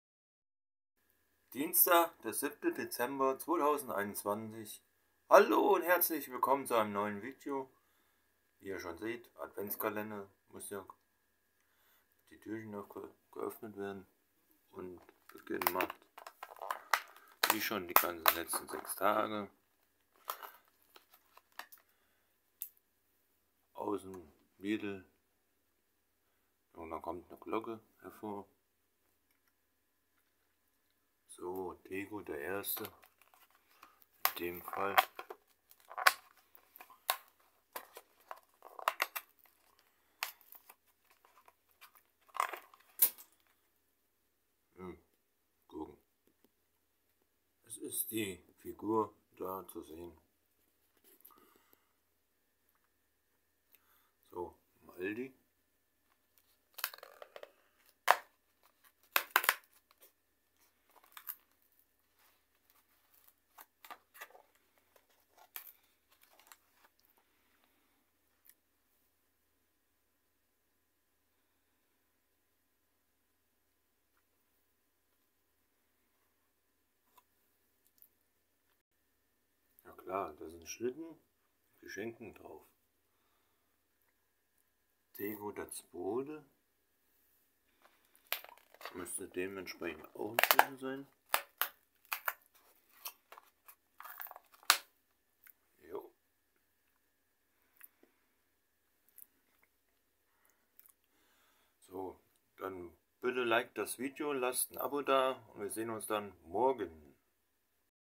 🍫 🍫 7. Türchen vom ADVENTSKALENDER 2021 geöffnet - OHA 🍫 🍫
Tja, nach dem gestrigen NIKOLAUStag stand auch am heutigen Dienstag das Öffnen vom Adventskalender auf der Agenda. 🤓 🤓 🤓 🤓

Oh, falls ich es noch nicht erwähnt im 1. VIDEO ) habe, meine 4 Adventskalender haben zusammen keine 5 € gekostet.

Jedenfalls gab es auch heute wieder was zu bestaunen. 👀 👀 👀 👀